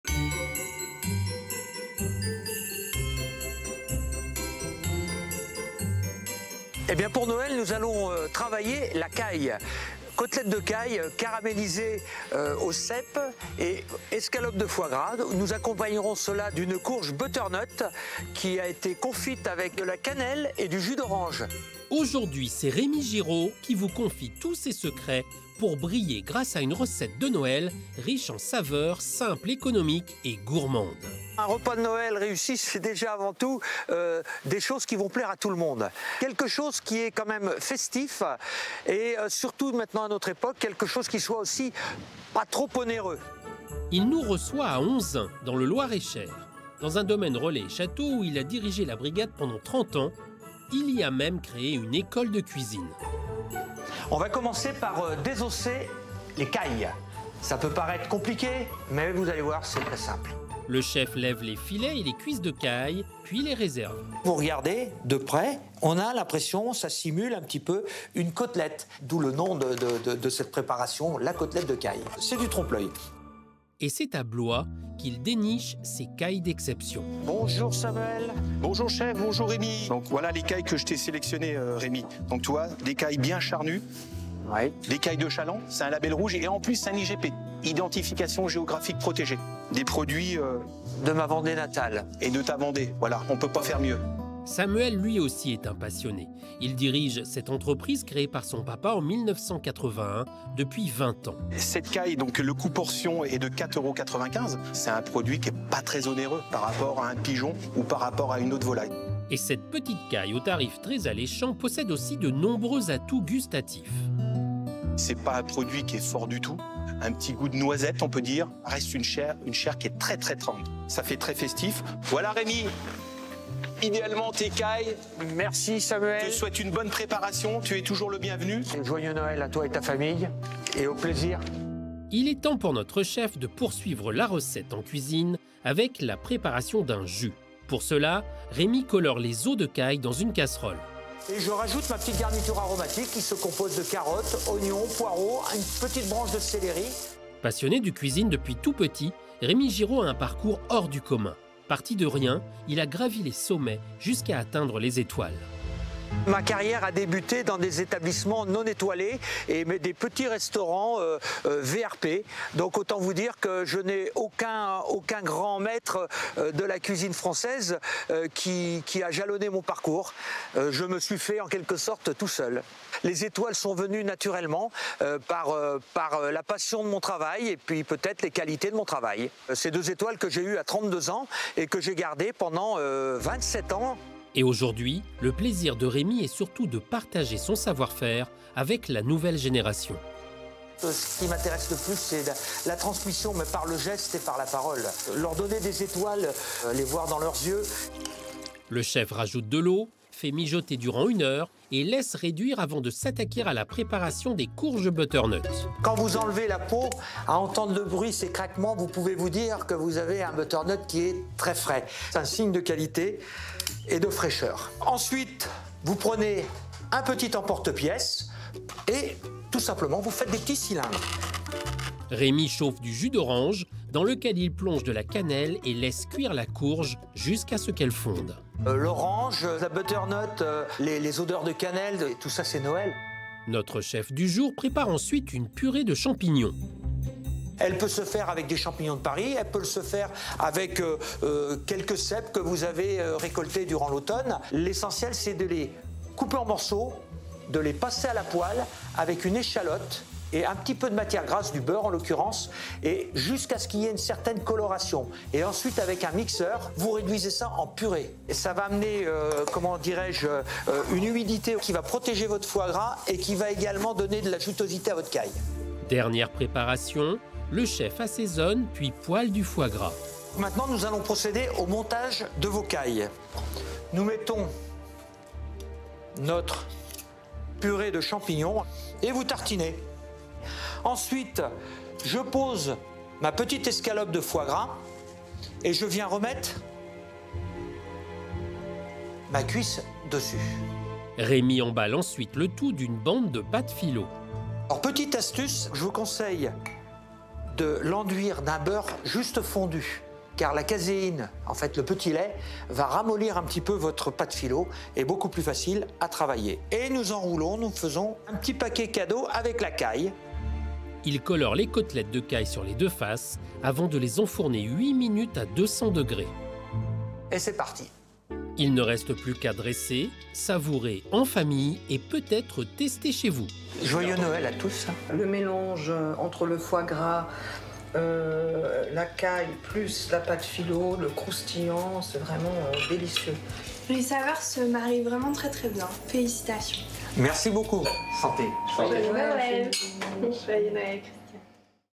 Table de fêtes : cailles, butternut et foie gras
Pour continuer sur notre lignée de recettes étoilées à petit prix, on vous propose aujourd’hui des cailles caramélisées accompagné de butternut et d’une touche de foie gras, Noël oblige ! C’est au chef Rémy Giraud qu’on doit ce plat de fête.

Ingrédients :

- Cailles
- Carottes
- Poireau
- Céleris
- Oignon
- Courges butternut
- Champignons
- Échalotes
- Foie gras

Recette :

1. Désosser les cailles, levez les filets et les cuisses des cailles et réservez
2. Colorez les os des cailles
3. Ajoutez la garnitures aromatiques et recouvrez-la d’eau, pour laisser réduire pendant 1h
4. Faites des cylindre avec les butternut et laissez-les cuire dans du jus d’orange et de la cannelle, laissez-cuire la courge jusqu’à ce qu’elle devienne fondantes
5. Coupez les champignons en morceaux, faites-les revenir avec une échalotes et du beurre jusqu’à ce qu’elles colorent, puis réduire en purée
6. Assaisonner et poêler le foie gras
7. Tartinez la purée de champignons sur les cailles, ajoutez l’escalope de foie gras et ajoutez la cuisse de caille. Emballez le tout dans une bande de pâte filo
8. Colorez le rouleau des deux face, enfournez 8 minutes à 200 degrés
9. Dressez c’est prêt !

Chef : Rémy Gireaud

RÉAGISSEZ
Hashtag